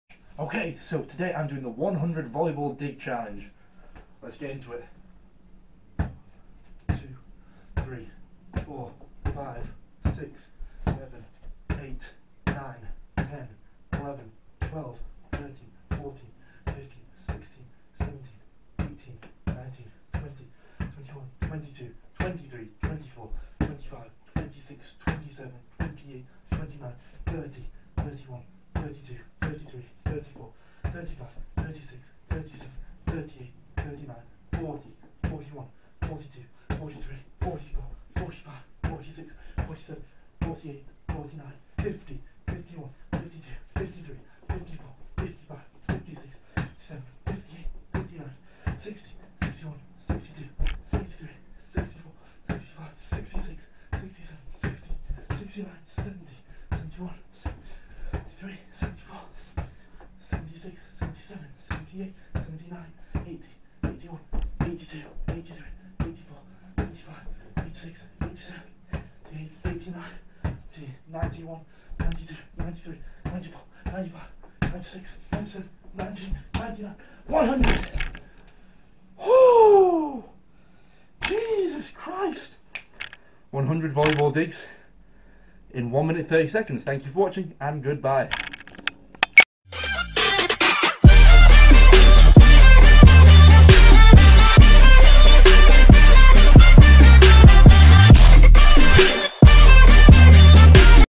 VOLLEYBALL 100 DIG CHALLENGE!!
I attempt 100 volleyball digs without dropping the ball once!